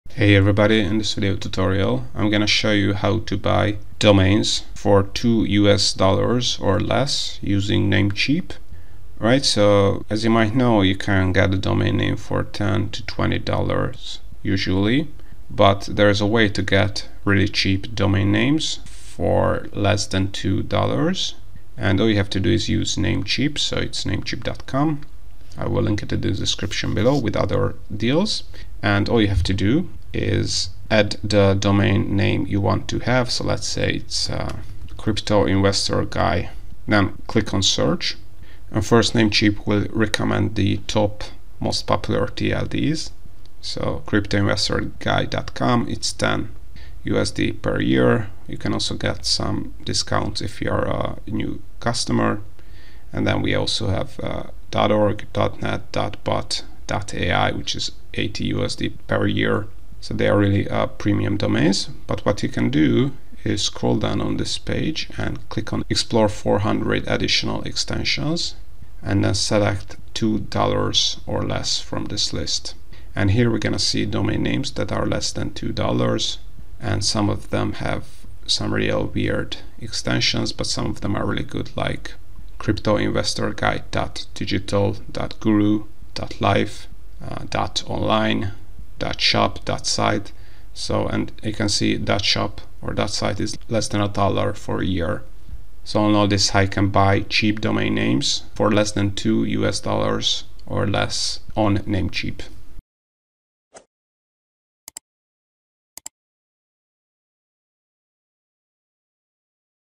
How To Buy Domains For $2 Or Less on Namecheap? [in 2025] (Cheap Domain Names)
Learn how to buy cheap domain names on Namecheap.

🤑 𝐒𝐚𝐯𝐞 𝐔𝐩 𝐓𝐨 𝟓𝟎% 𝐎𝐧 𝐘𝐨𝐮𝐫 𝐃𝐨𝐦𝐚𝐢𝐧 𝐑𝐞𝐧𝐞𝐰𝐚𝐥 𝐅𝐞𝐞𝐬 𝐖𝐢𝐭𝐡 𝐃𝐲𝐧𝐚𝐝𝐨𝐭

🆓 𝐂𝐥𝐚𝐢𝐦 𝐘𝐨𝐮𝐫 𝗙𝗿𝗲𝗲 𝗔𝘂𝗱𝗶𝗼𝗯𝗼𝗼𝗸 𝗪𝗶𝘁𝗵 𝗔𝘂𝗱𝗶𝗯𝗹𝗲